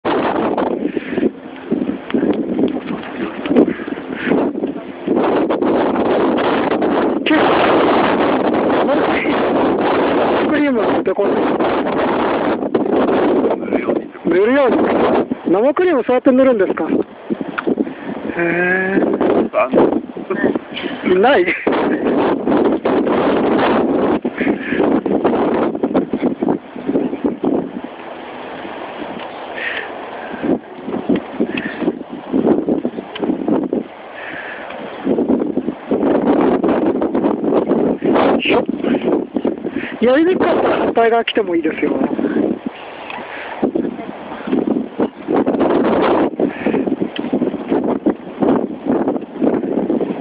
今度はママと一緒 20120211
生クリームをケーキに塗るみたい？、ママと一緒に立てた畝をならしています。